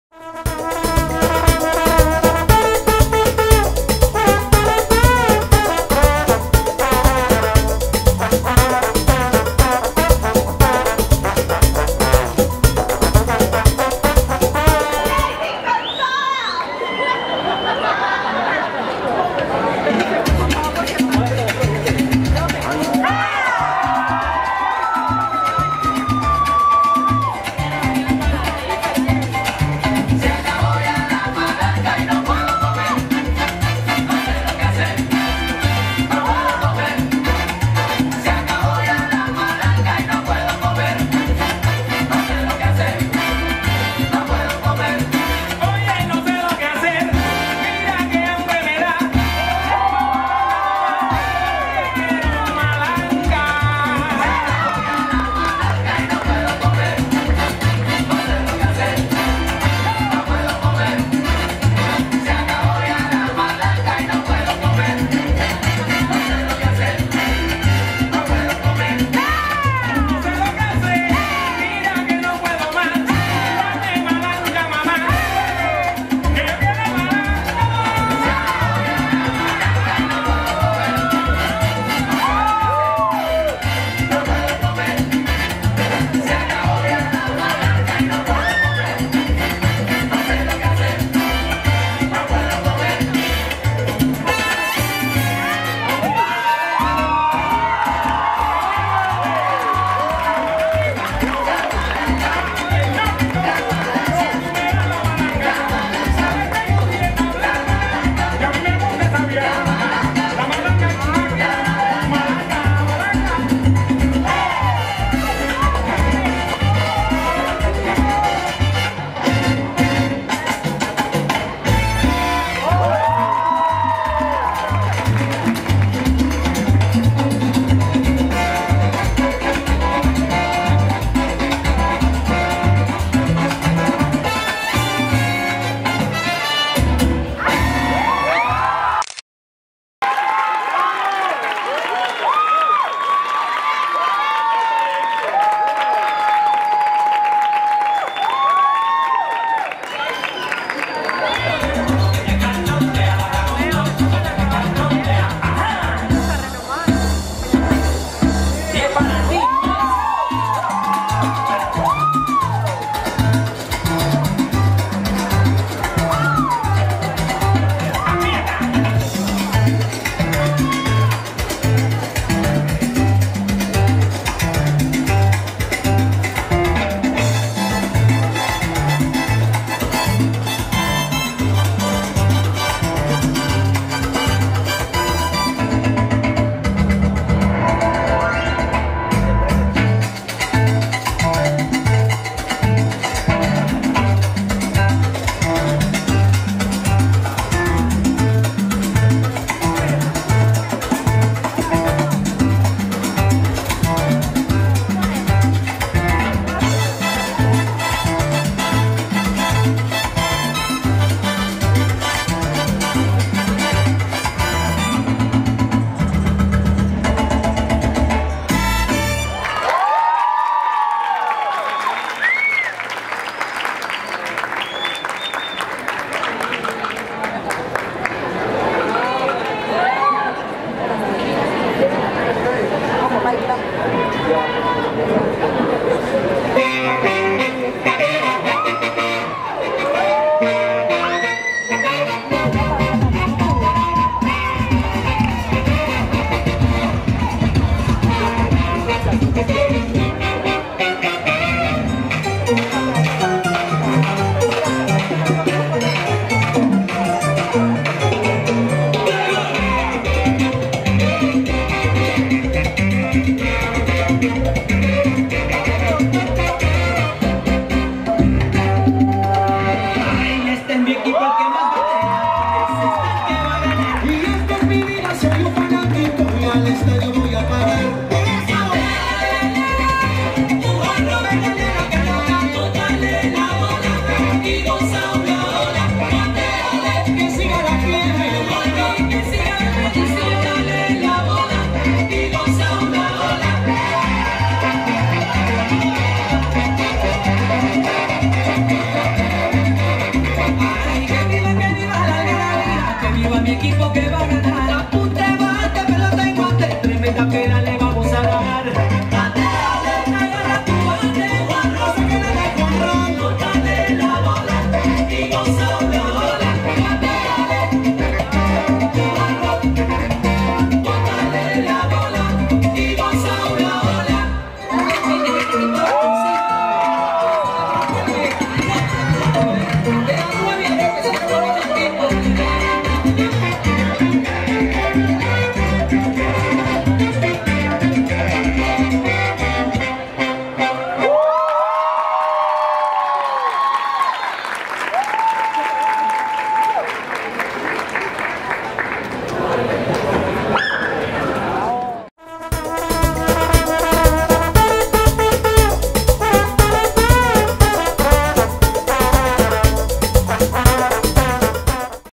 Salsa U.N. Día Internacional de la Danza
Presentación del grupo de Proyección Salsa U.N. en la celebración del Día Internacional de la Danza en la Universidad Nacional de Colombia, Sede Medellín.